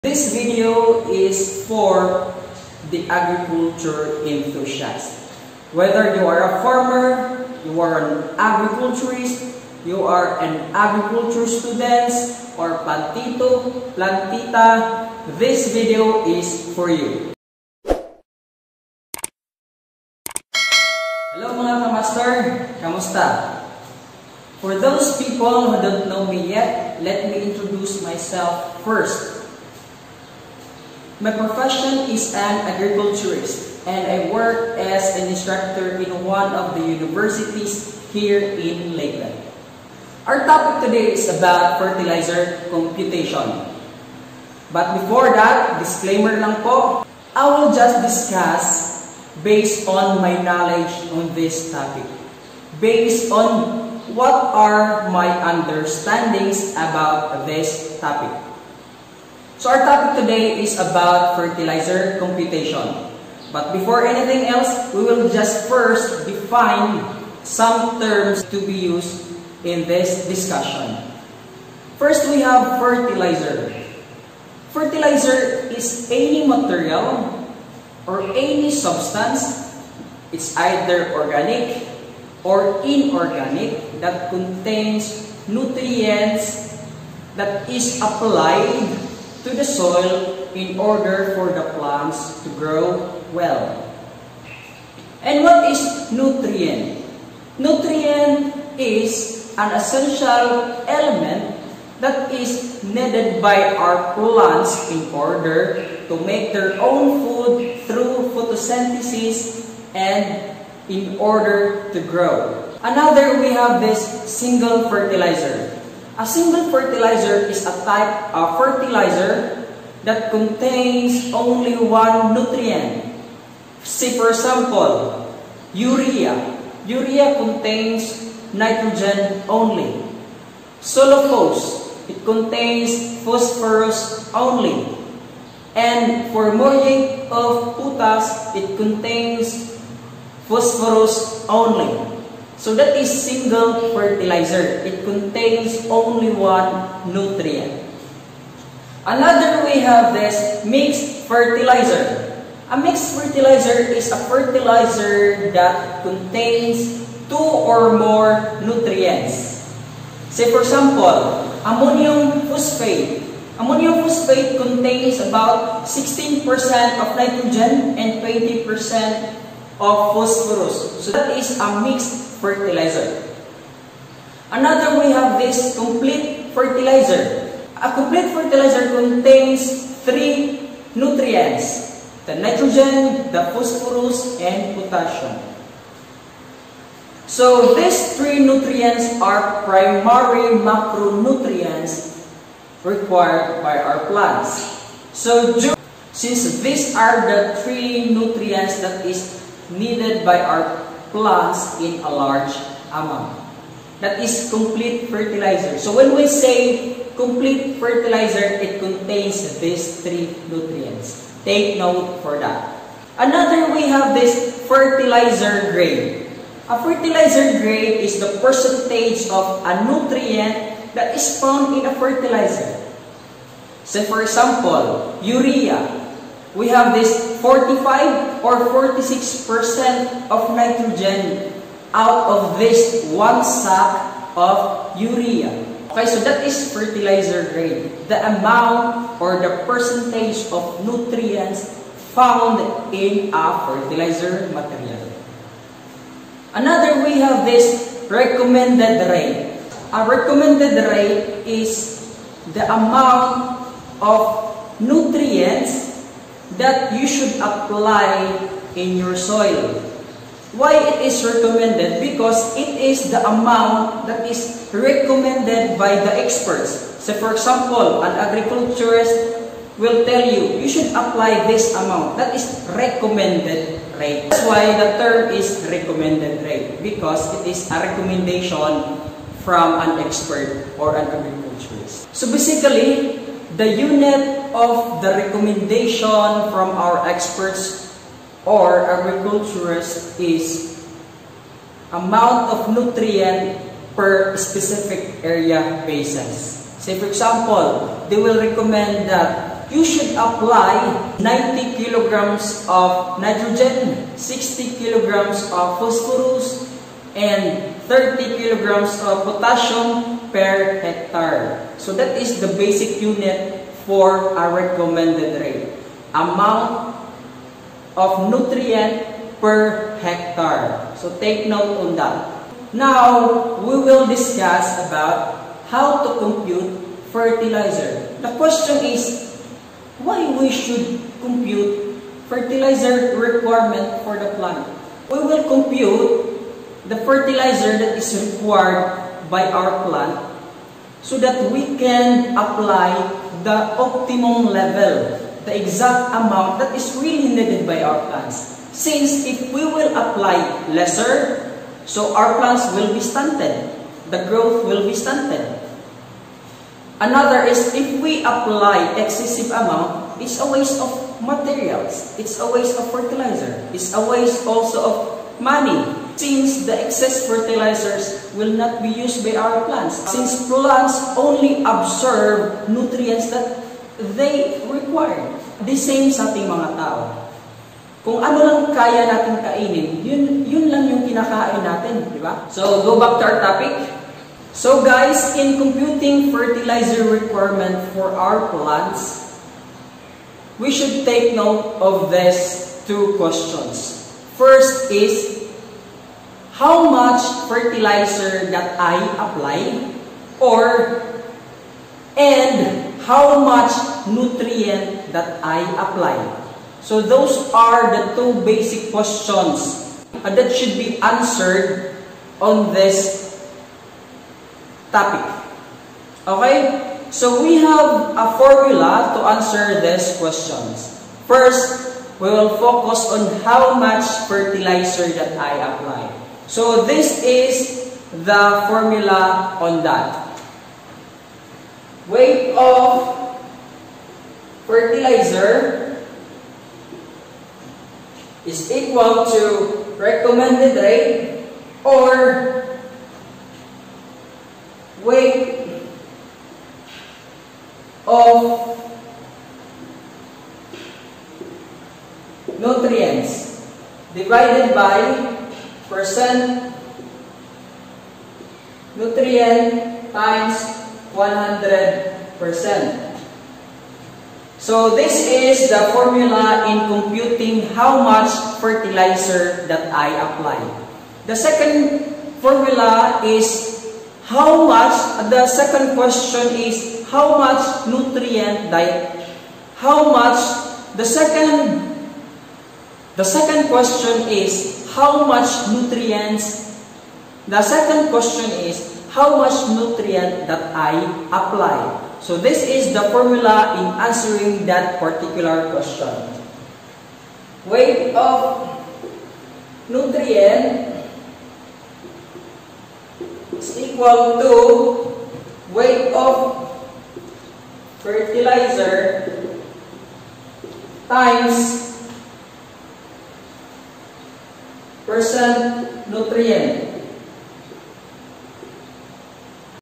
Part 1. Fertilizer Computation ( Definition of Terms/Importance of Fertilizer Computation)
The part 1 talks about the definition of terms and importance of fertilizer Computation.